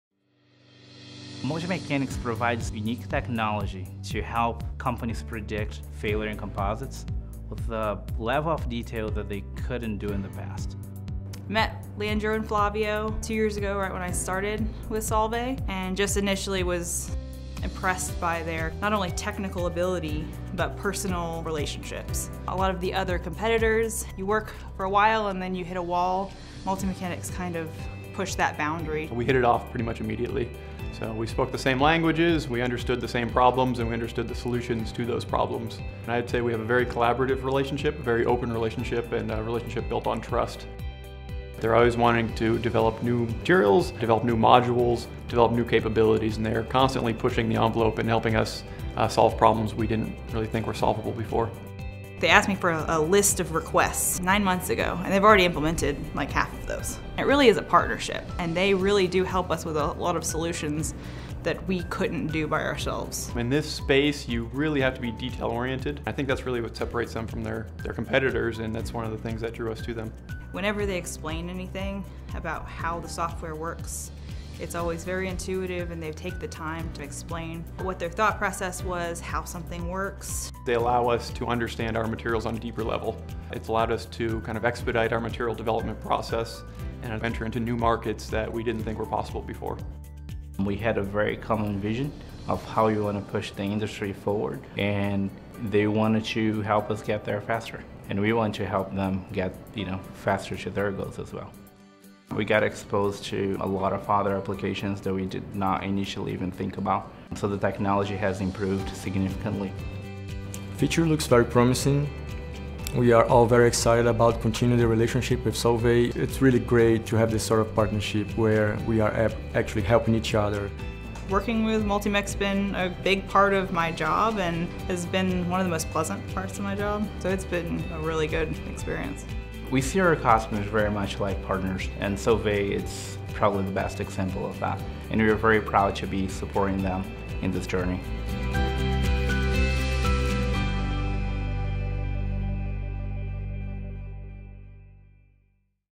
Solvay Testimonial Video - MultiMechanics
MultiMechanics allows Solvay to understand their materials on a deeper level, expedite the material development process, and enter into new markets.